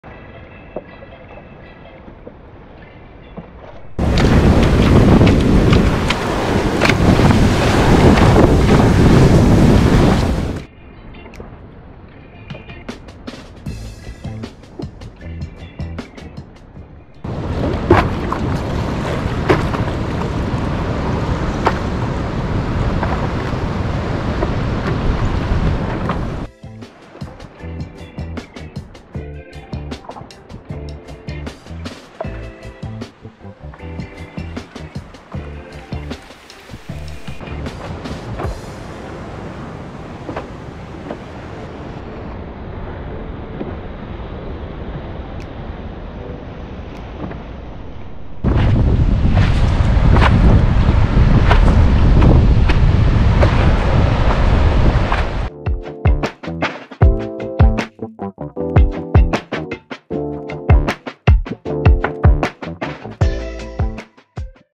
Hawaii Severe Weather Thanksgiving 2022 Kailua Bay Waves Sea Turtles
Updated to include 11/26/22 footage. Here is a comparison between a calm day and rougher conditions under a "small craft advisory" in Kailua Bay.  A high surf warning is in effect for the North Shore. November 25-26, 2022.